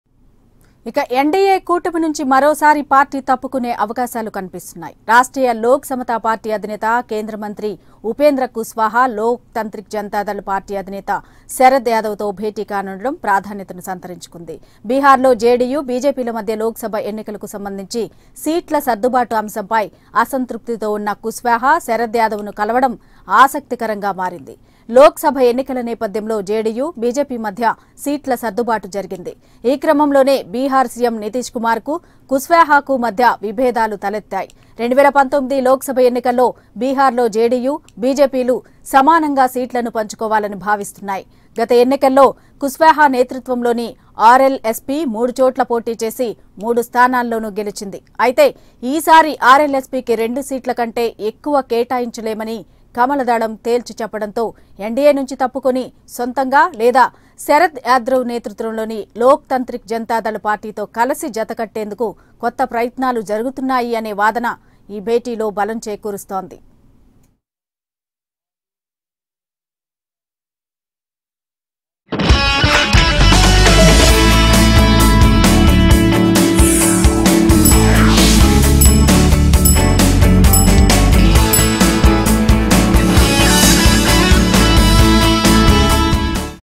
Will RLSP Differ From NDA Over Seats Issue? | AP24x7
Will RLSP differ from NDA over seat issue? BJP is decided to give only 2 seats to RLSP and there is a chance for a new alliance in Bihar. RLSP president and minister Kuswaha met with Sarad Yadav.